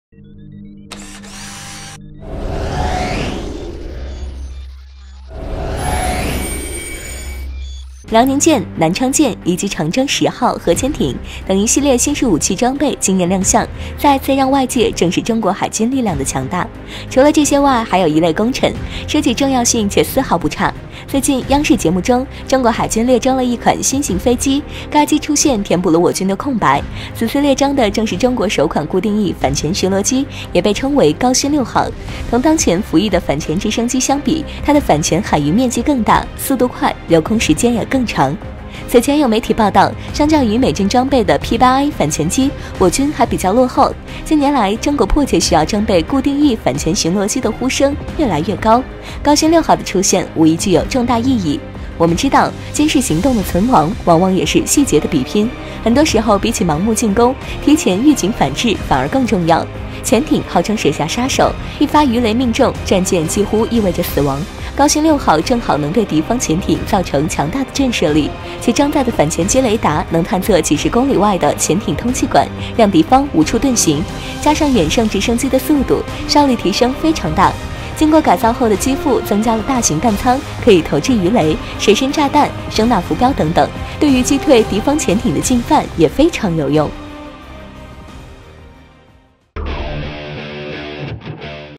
首款！中国海军又列装一新型飞机，该机的出现具有重大意义
最近，央视节目中，中国海军列装了一款新型飞机，该机出现填补了我军的空白。（叮允） 我們的影片會遵循公平、尊重的原則，確保不會包含任何仇恨言論的元素。